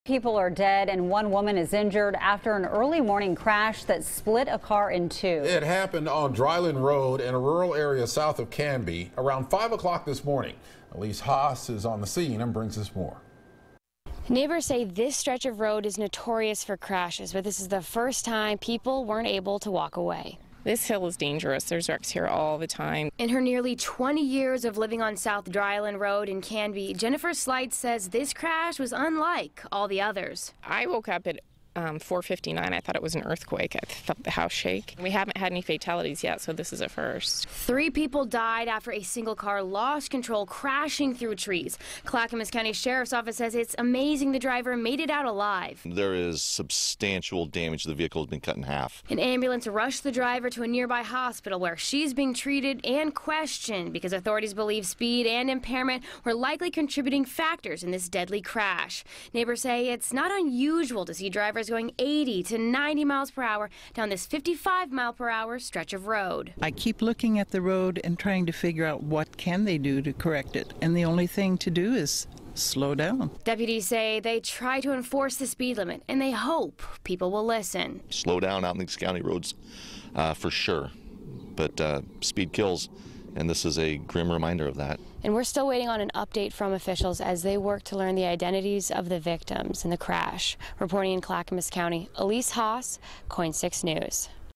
3 passengers in their 20s die in Canby crash
A single-car crash early Tuesday in Canby killed 3 passengers. But the driver, a woman, survived.